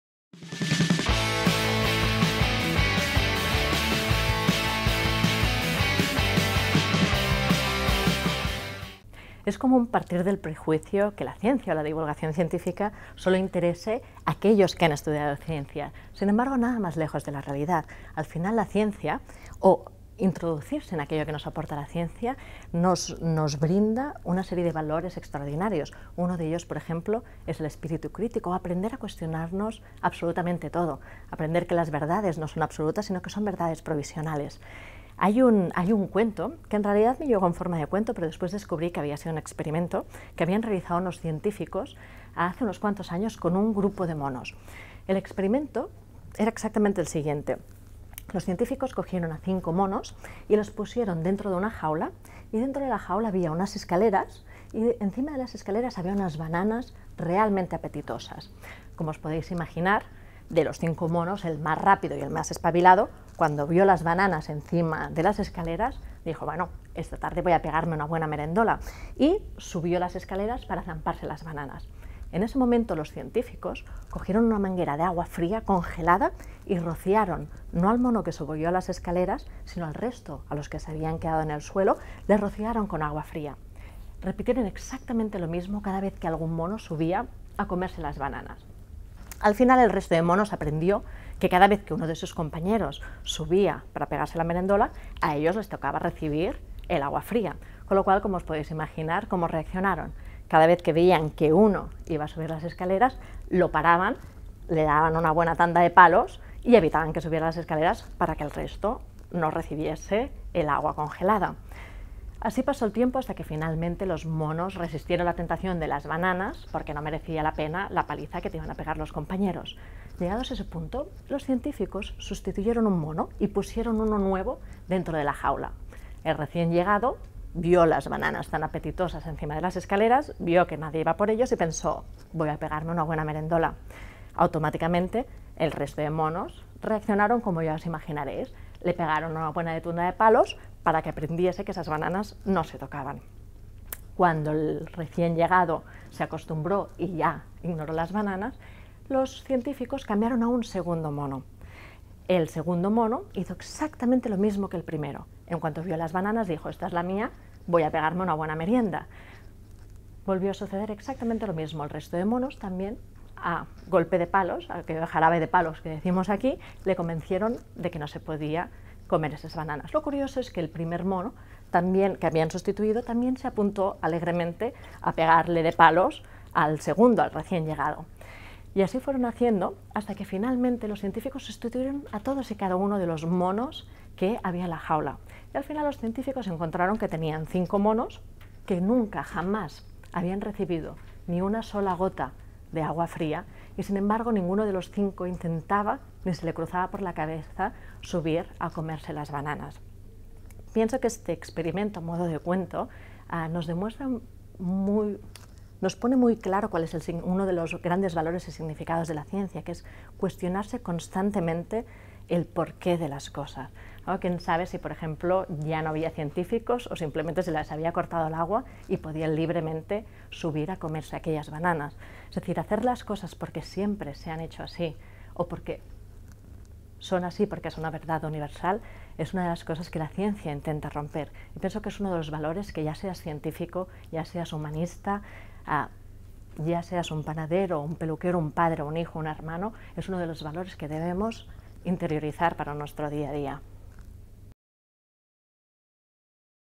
Descubre el EXPERIMENTO de los MONOS y las BANANAS
Sonia Fernández-Vidal nos cuenta la historia de un experimento muy original que tiene como protagonistas a los monos y las bananas. ¿Cuento o experimento? Sea lo que sea... ¡no te lo pierdas!

Sonia Fernández-Vidal es una física autora de bestsellers de divulgación científica.